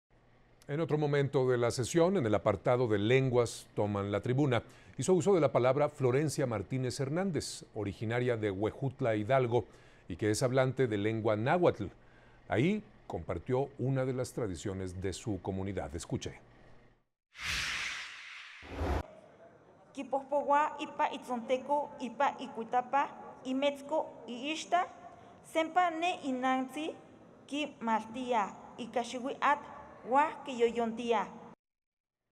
Comparte tradiciones de comunidad indígena
En el apartado de "Lenguas toman la tribuna", hizo uso de la palabra Florencia Martínez Hernández, originara de Huejutla, Hidalgo, y que es hablante de la lengua Náhuatl, quien compartió una de las tradiciones de su comunidad…